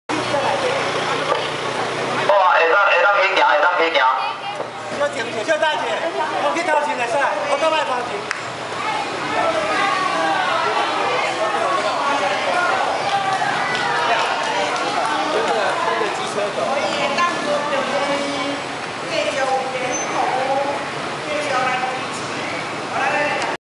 991117呂秀蓮陪同新北市何淑峰議員車隊掃街02剛上車
99.11.17下午.呂秀蓮前副總統陪同新北市新五泰林選區何淑峰議員新莊市車隊掃街拜票02剛上車
臉書相關照片煩請參閱